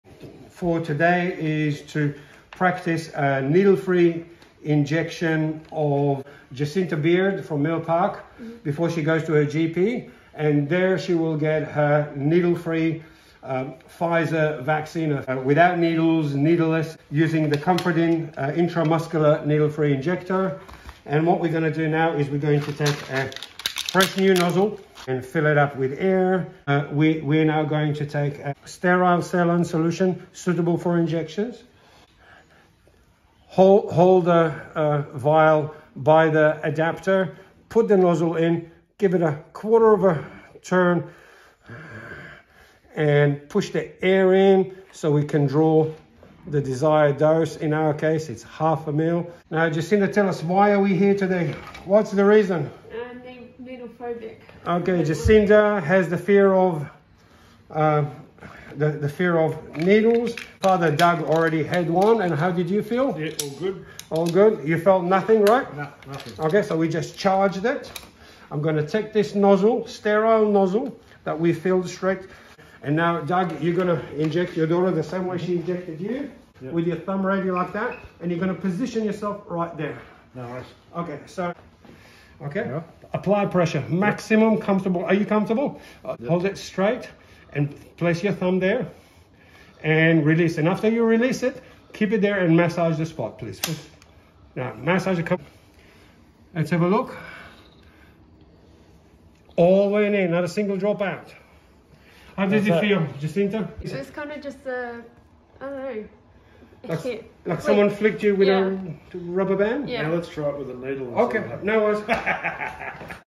Coronavirus Pfizer vaccine without needles in Melbourne for a girl with needle phobia
First time in Australia coronavirus Pfizer vaccine without needles.

Today at 11 am in Milll Park Victoria,Australia,
a lovely young lady with needle phobia received a Covid19 Pfizer vaccine without needles, in a local medical centre by a nurse, using the Comfort-in™️needle-free intramuscular injector.
Jacinta didn’t have any type of injections since she was 6 years old due to her needle phobia until today.
This video was the practice Injction at home before the vaccination took place at her local medical centre.